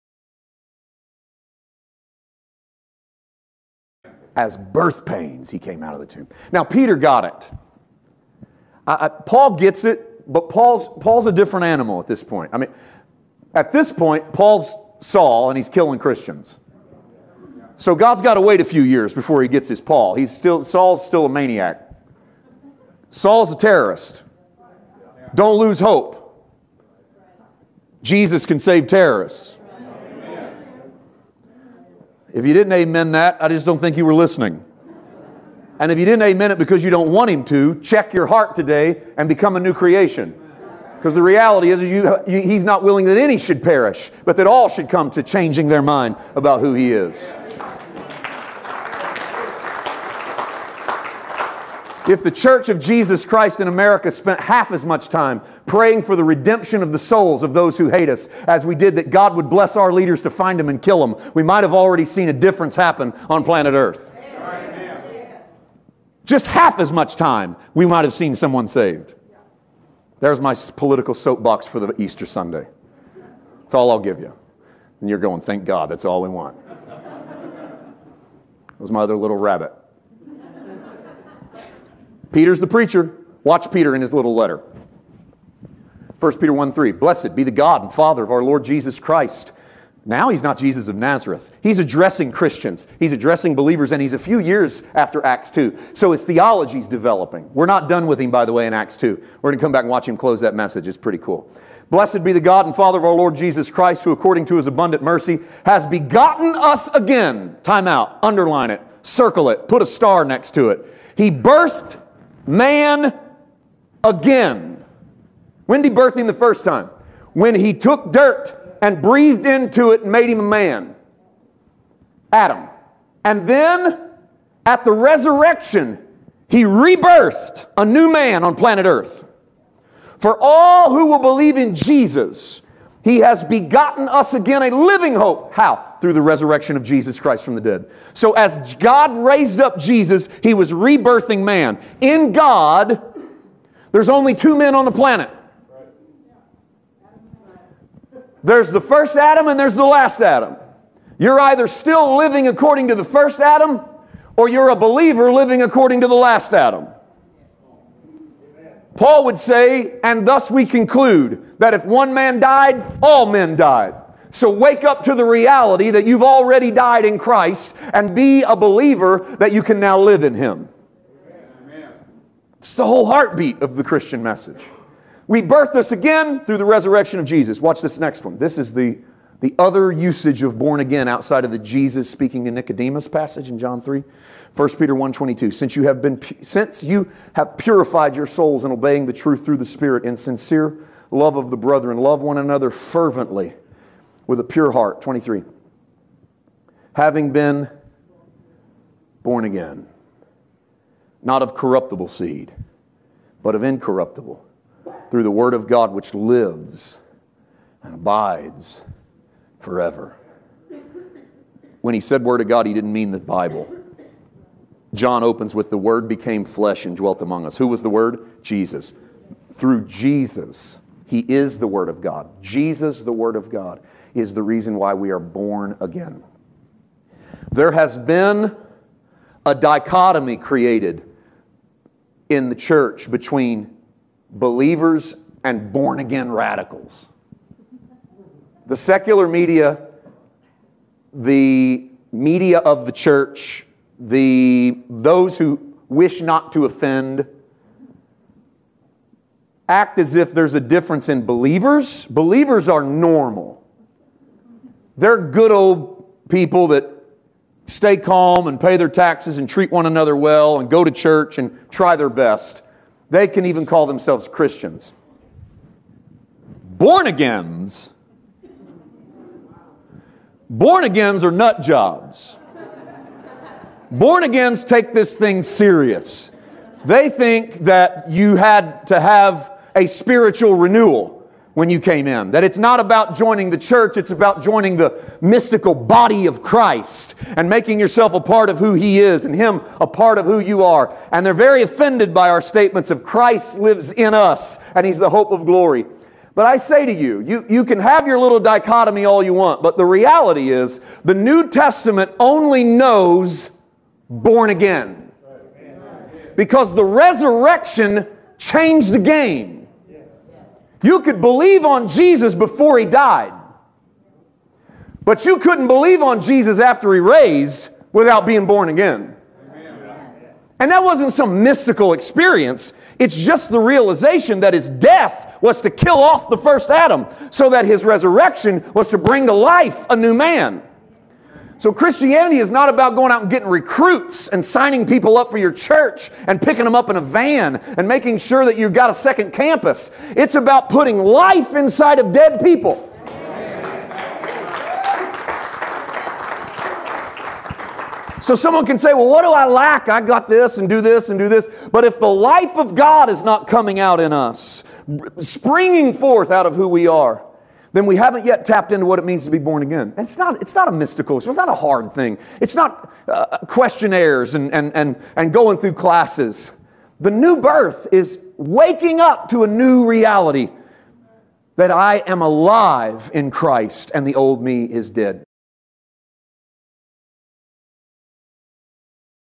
Nuggets of Grace: The Reason We Are Born Again
Paul White teaching from Paul White Ministries. For more information on our ministry, visit us at PaulWhiteMinistries.org.